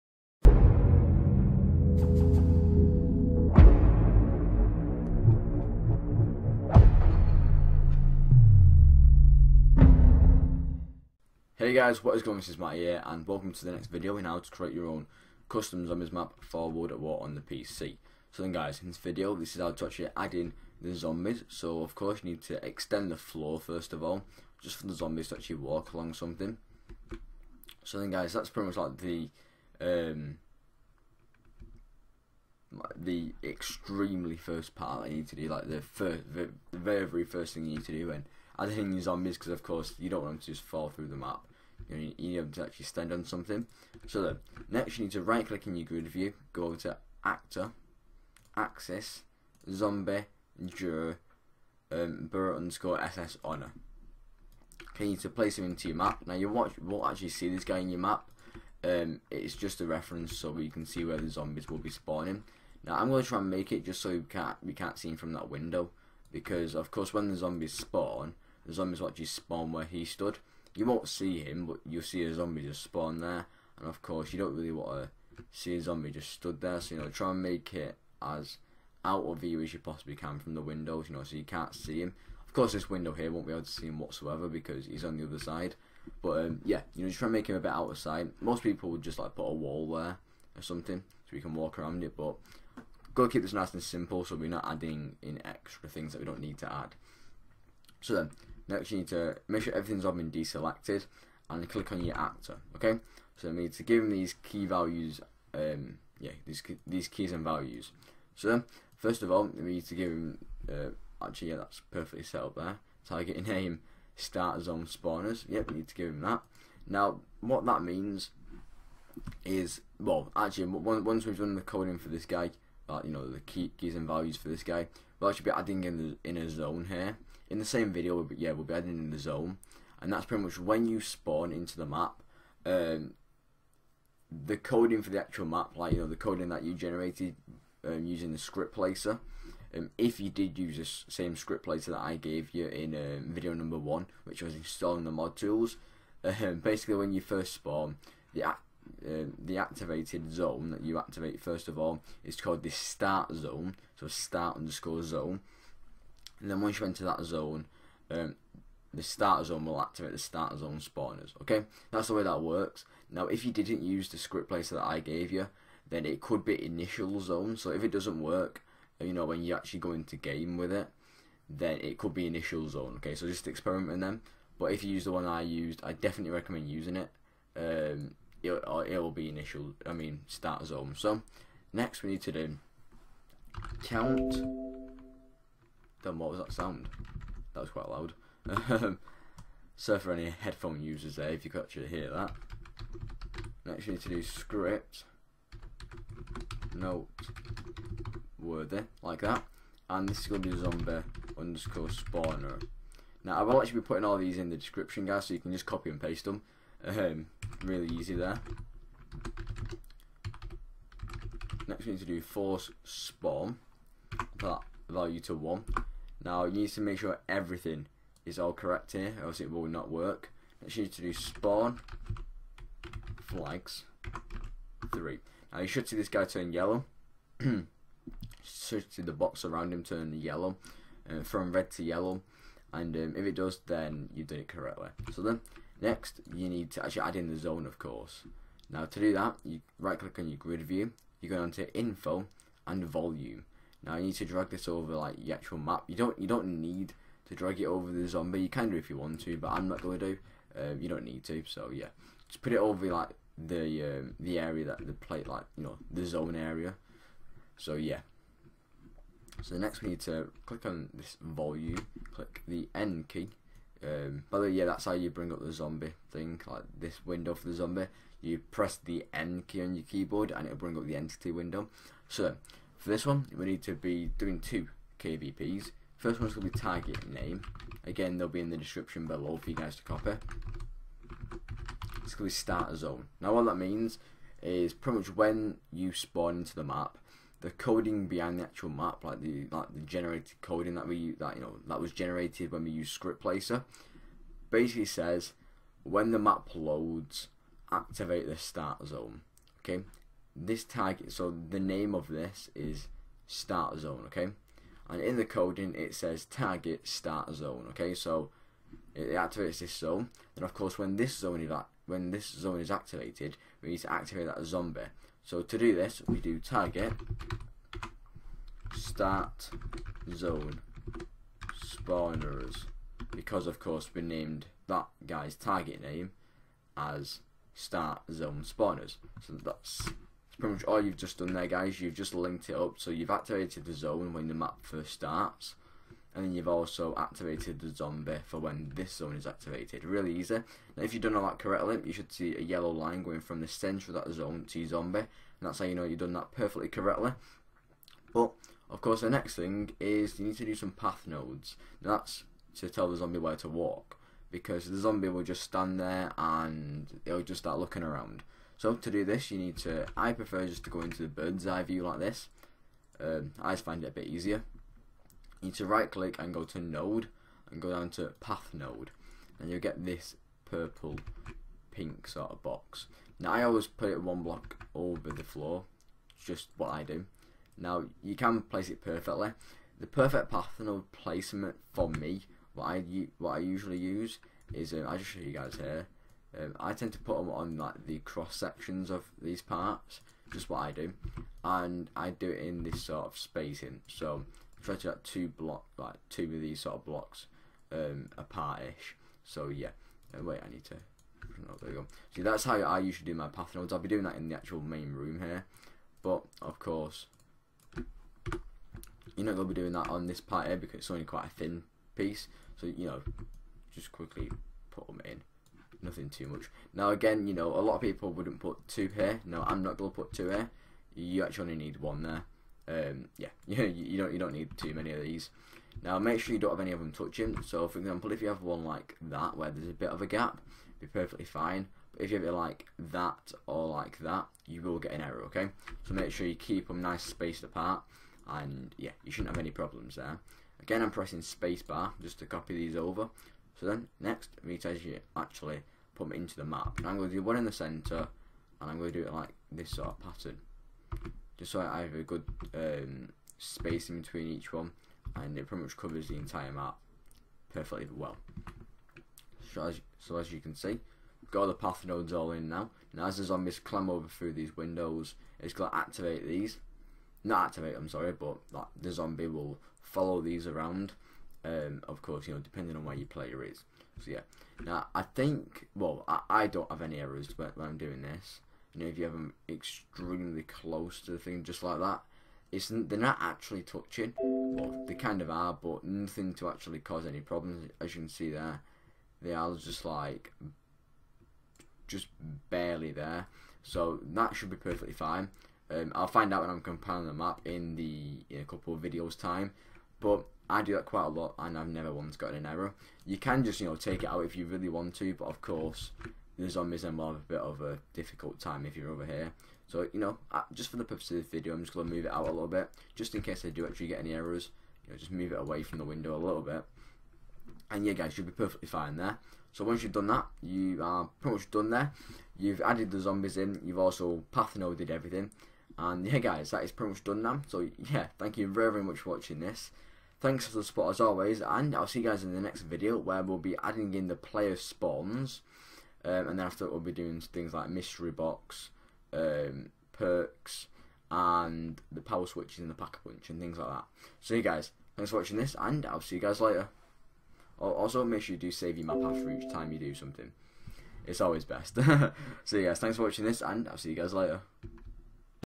Making A Custom Zombies Map - Adding Zombies, The Start Zone, and Path Nodes
To open the KvP/code window press the 'N' key.

KvP's:

Zombies/actor:

targetname
start_zone_spawners

count
9999

script_noteworthy
zombie_spawner

forcespawn
1

spawnflags
3

Zone:

targetname
start_zone

target
start_zone_spawners